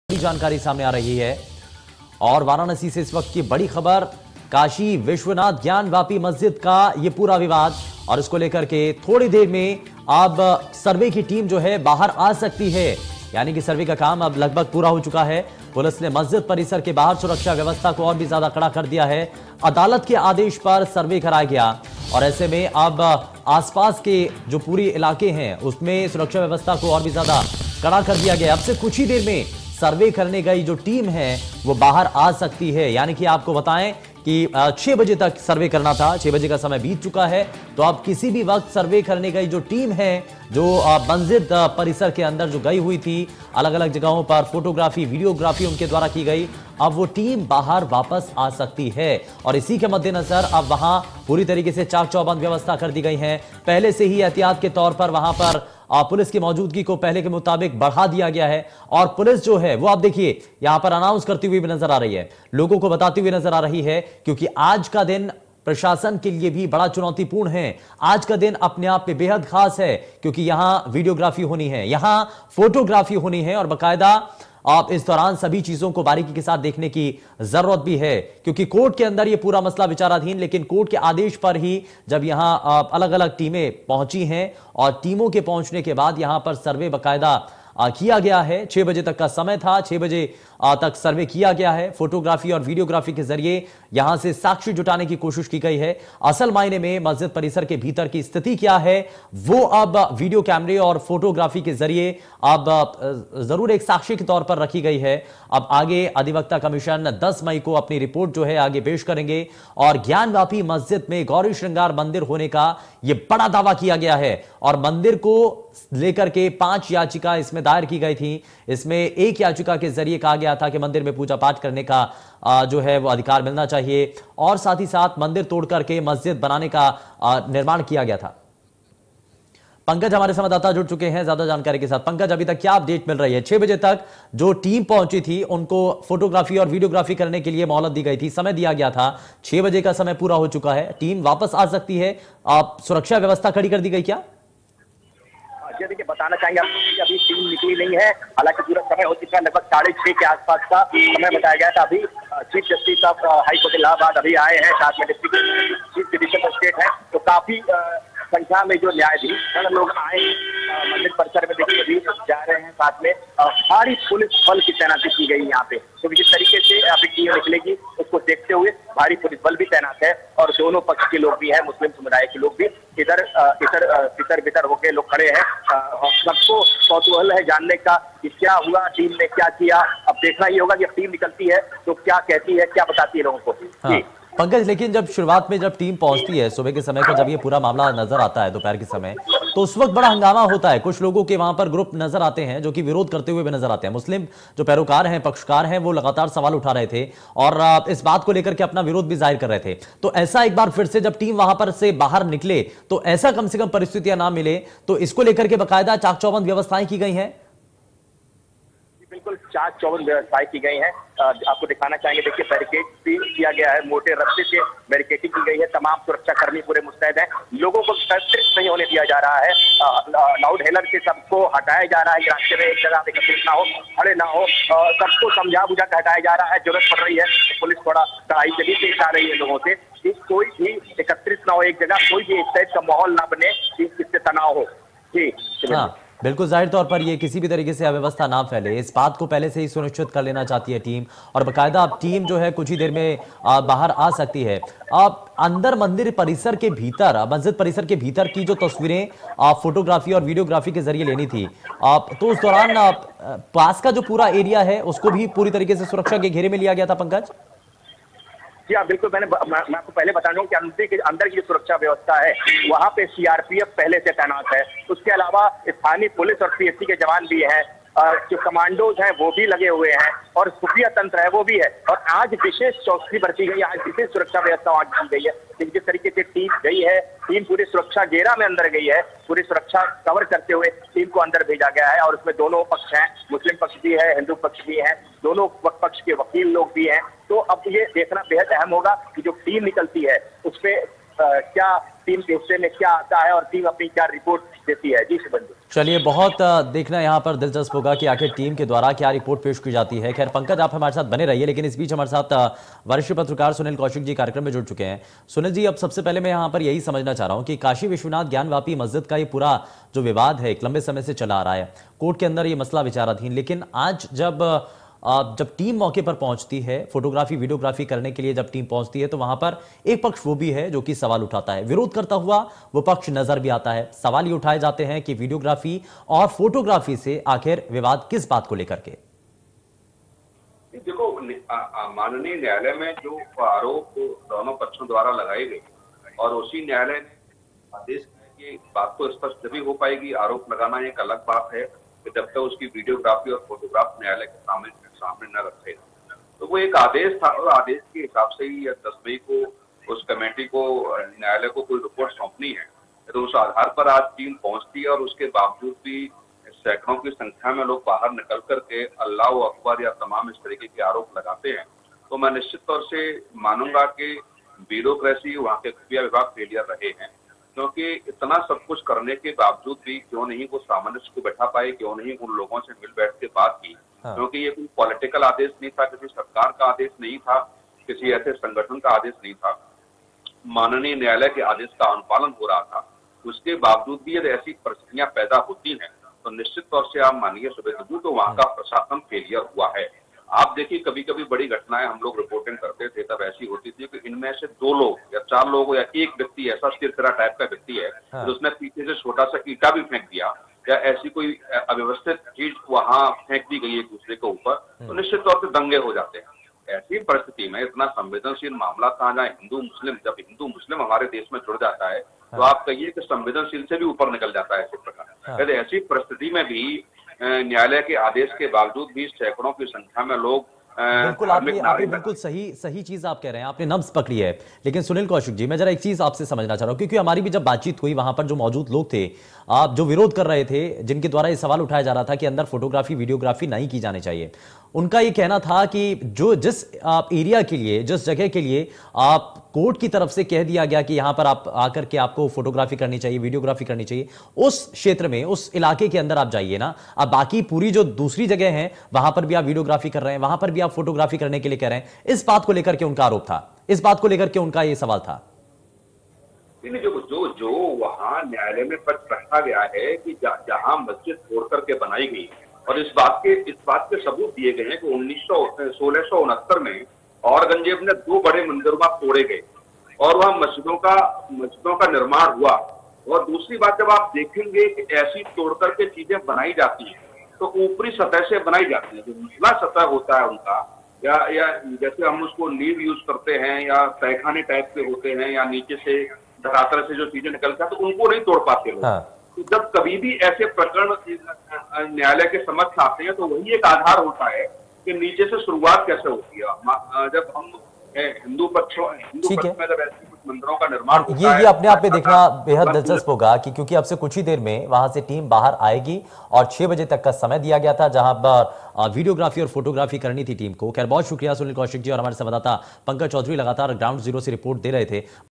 ज्ञानवापी मस्जिद : श्रृंगार गौरी से क्या शुरू हुआ Mission काशी अध्याय ? जानिए क्या है मामला.
उत्तर प्रदेश स्थित वाराणसी में ज्ञानवापी परिसर में वीडियोग्राफी के विरोध में नारेबाजी हुई है. मिली जानकारी के अनुसार मौके पर मौजूद कुछ लोगों ने नारेबाजी की. यह वीडियोग्राफी और सर्वे अदालत के आदेश पर कराया जा रहा है. वहीं नारेबाजी और हंगामे के बाद पुलिस ने इलाके में गश्त बढ़ा दी है | India News UPUK